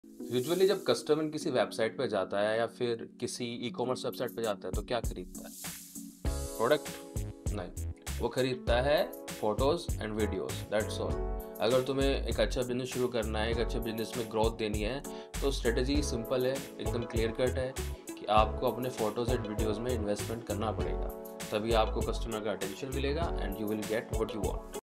Why photos & Videos are important for Business? | The Video Mistrii Co. | Best Production house
Photos and videos are essential for businesses because they grab attention, tell stories effectively, showcase products, boost social media engagement, improve SEO, demonstrate expertise, and encourage sharing, ultimately helping businesses connect with customers and grow.